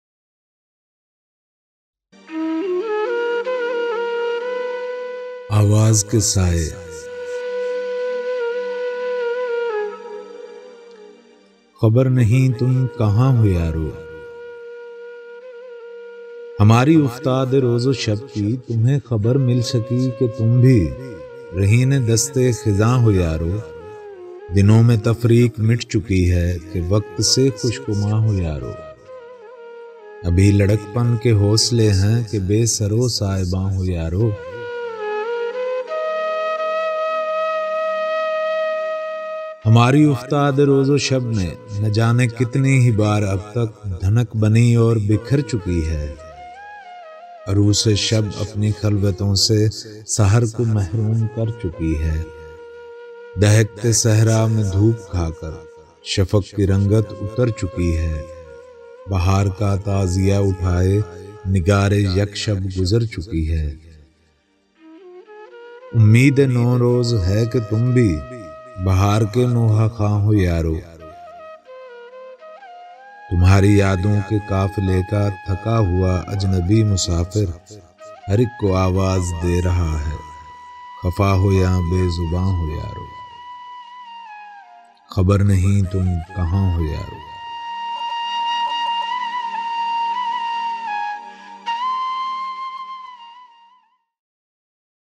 Awaz K Saaye|Mustafa Zaidi|Sukhan Shnas
Mustafa Zaidi  was a Pakistani Urdu poet and a civil servant.He also wrote under his pen-name Tegh Allahabadi. His initial poetry was romantic in nature. At the age of 17, published his first collection of poetry Zanjeeren in 1949, followed by, Zangeerein (1949), Roshni (1950), Shehr-e-Azar (City of Idol Worshippers; 1958), Mauj Meri Sadaf Sadaf (1960), Gareban (1964), Qaba-e-Saaz (1967) and Koh-e-Nida (1971) (published posthumously). His complete work was published as Kulliyaat-i-Mustafa Zaidi posthumously.
ḳhabar nahīñ tum kahāñ ho yaaro

hamārī uftād-e-roz-o-shab kī

tumheñ ḳhabar mil sakī, ki tum bhī

rahīn-e-dast-e-ḳhizāñ ho yaaro

dinoñ meñ tafrīq miT chukī hai

ki vaqt se ḳhush-gumāñ ho yaaro

abhī laḌakpan ke hausle haiñ

ki be-sar-o-sā.ebāñ ho yaaro

hamārī uftād-e-roz-o-shab meñ

na jaane kitnī hī baar ab tak

dhanak banī aur bikhar chukī hai

urūs-e-shab apnī ḳhalvatoñ se

sahar ko mahrūm kar chukī hai

dahakte sahrā meñ dhuup khā kar

shafaq kī rañgat utar chukī hai

bahār kā ta.aziya uThā.e

nigār-e-yak-shab guzar chukī hai

umīd-e-nau-roz hai ki tum bhī

bahār ke nauha-ḳhvāñ ho yaaro

tumhārī yādoñ ke qāfile kā

thakā huā ajnabī musāfir

har ik ko āvāz de rahā hai

ḳhafā ho yā be-zabāñ ho yaaro